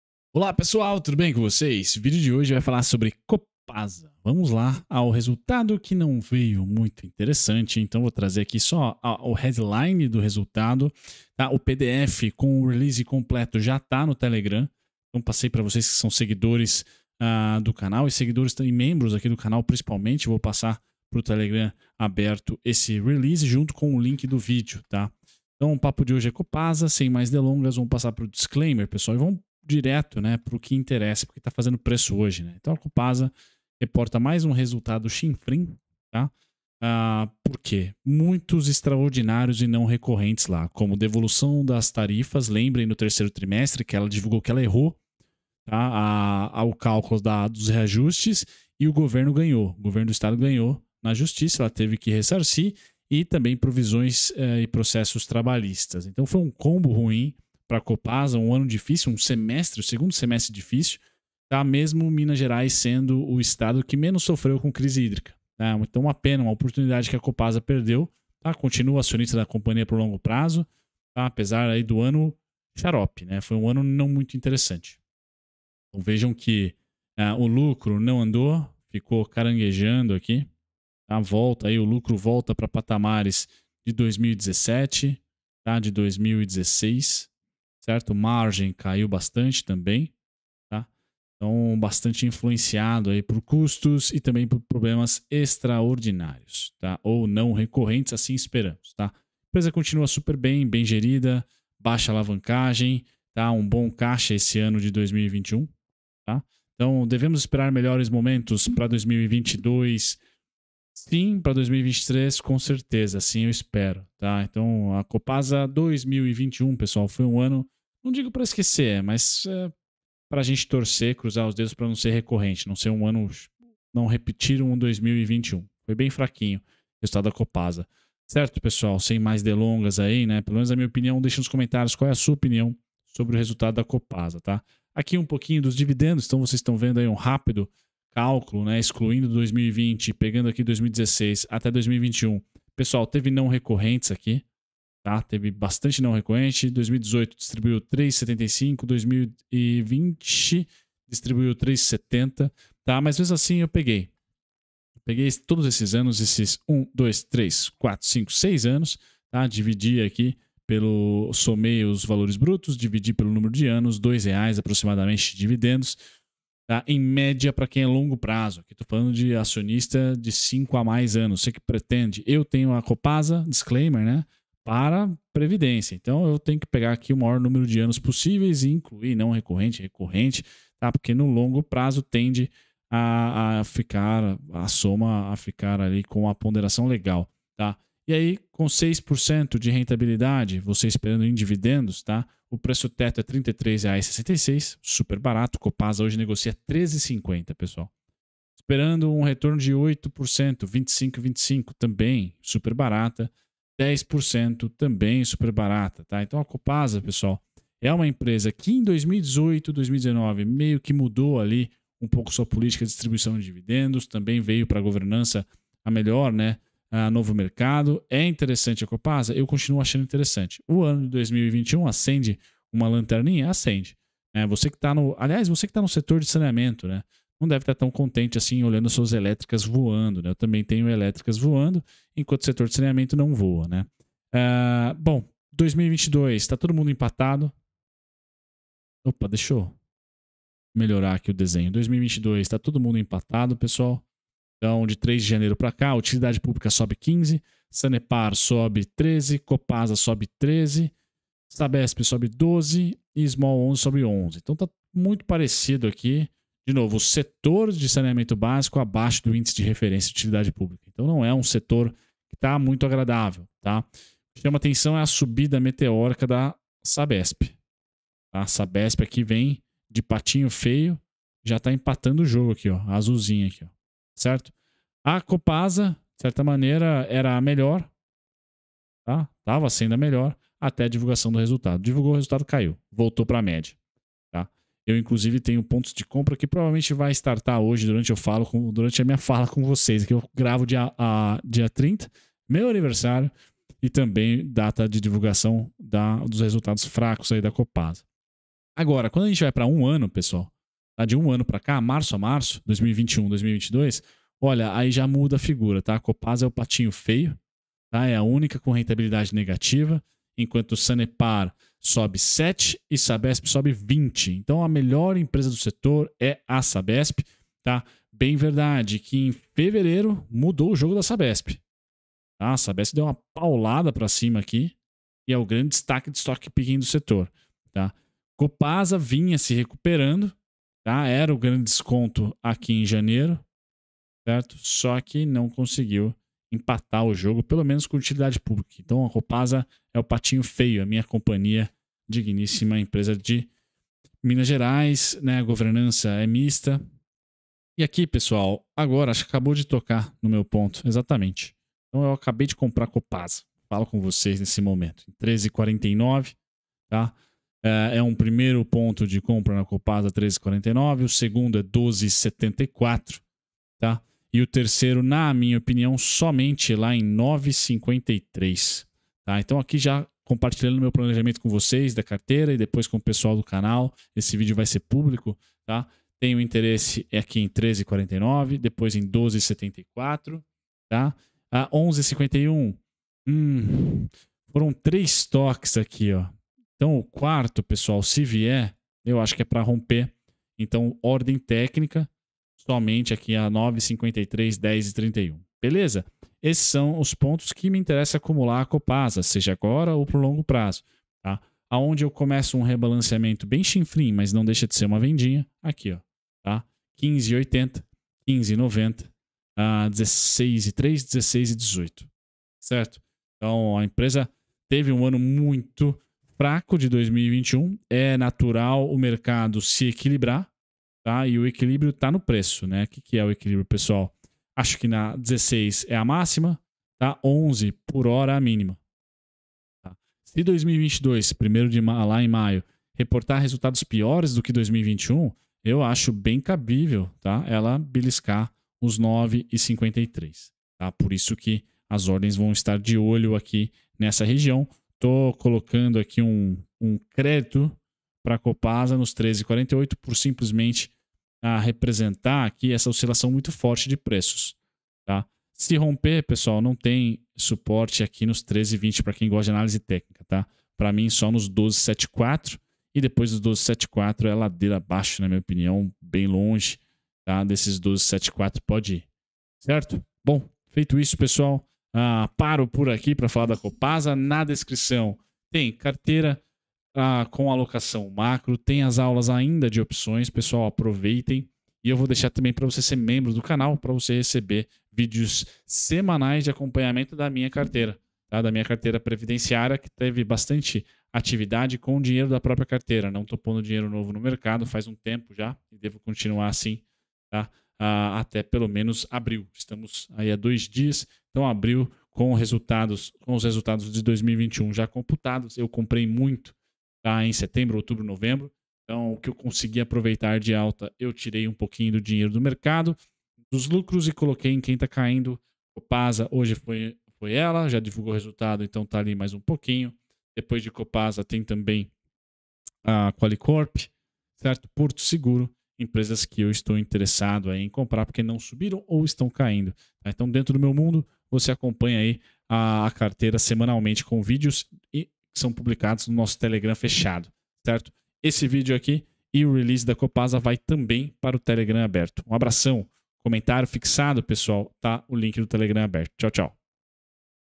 COPASA VAI CAIR ATÉ... ANÁLISE RESULTADOS, DIVIDENDOS E PREÇO TETO CSMG3
00:00 Seja bem vindo ao Clube dos Dividendos
00:20 Hub Conecta Trade Ideias ao vivo
00:31 Cenário Ibovespa, Dólar e Macro
00:40 Aprenda As Bases da Análise Técnica
01:05 Análise Resultado e Fundamentos da Copasa (csmg3)
02:03 Dividendos da Copasa (csmg3) e Cálculo Preço Teto
04:00 Rentabilidade Copasa e Setor de Saneamento 2022 (jan-mar) e 12 meses (março / março)
05:50 Copasa (csmg3) Análise Técnica

Carteiras Fundos Imobiliários e Investimentos Otimizado em Renda Fixa